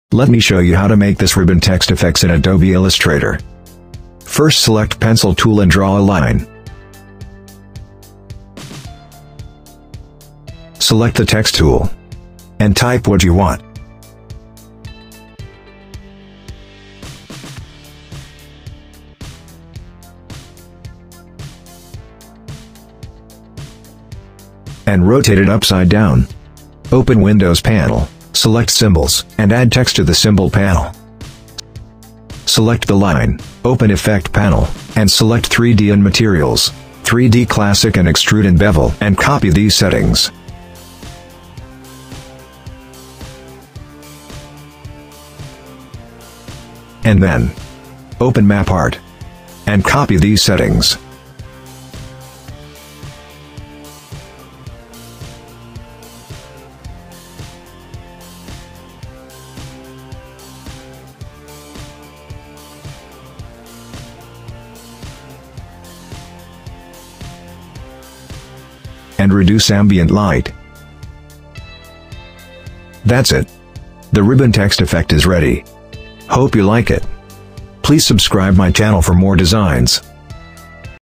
How to Create Ribbon Text Effect in Adobe illustrator | Illustrator Tutorial
Ckgraphics